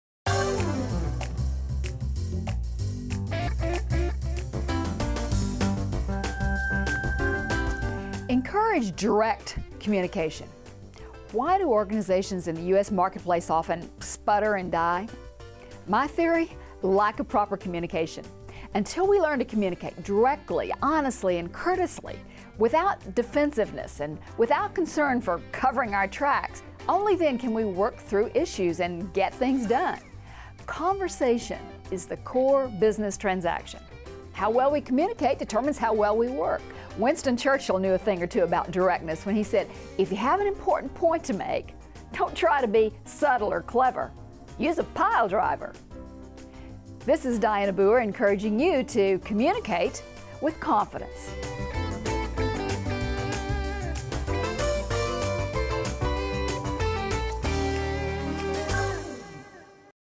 Dianna Booher's Communication Tips: Encourage Direct Communication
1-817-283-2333 Executive Communication Expert Dianna Booher. Communication expert Dianna Booher shares her insights about the most important asset on your balance sheet—direct communication!